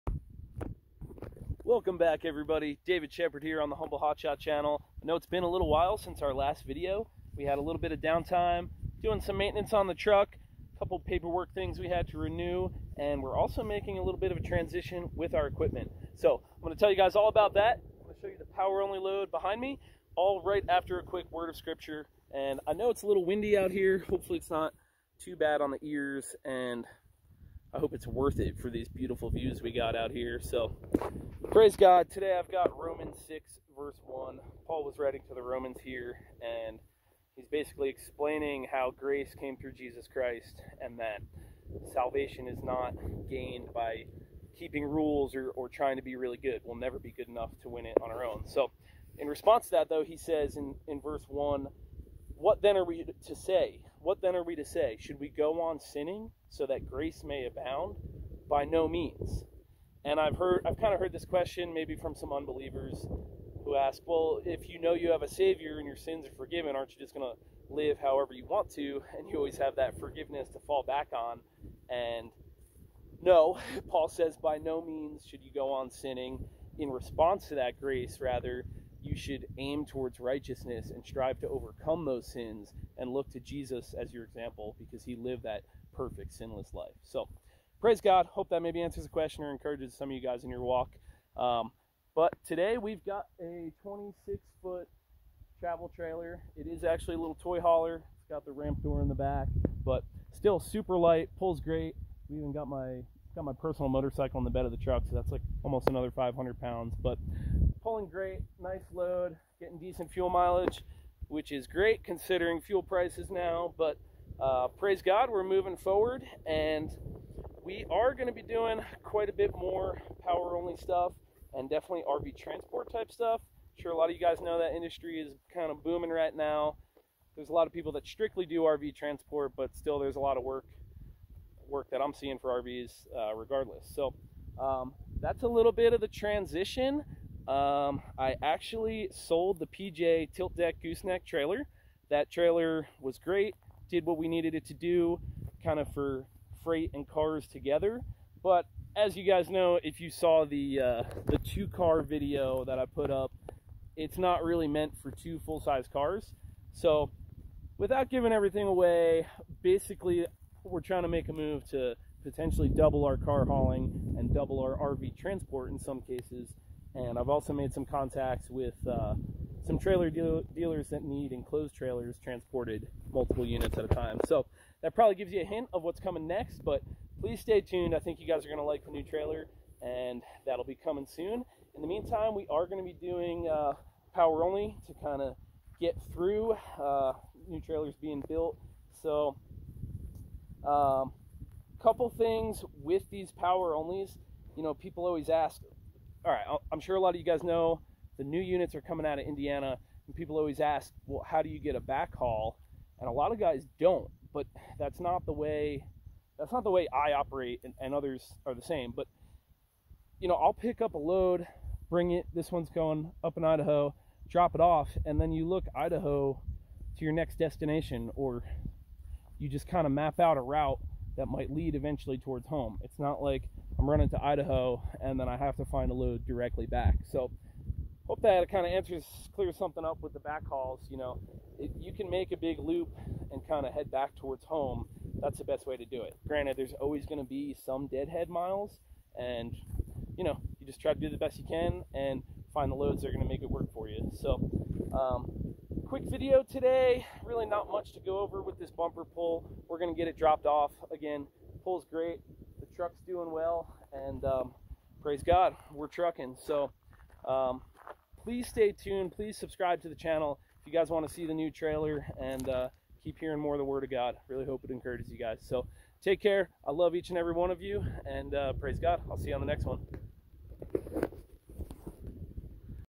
RV Transport, Sold the PJ, Power Only Hotshot, & future plans!
RV transport and other power only moves ! God Bless.
Romans 6:1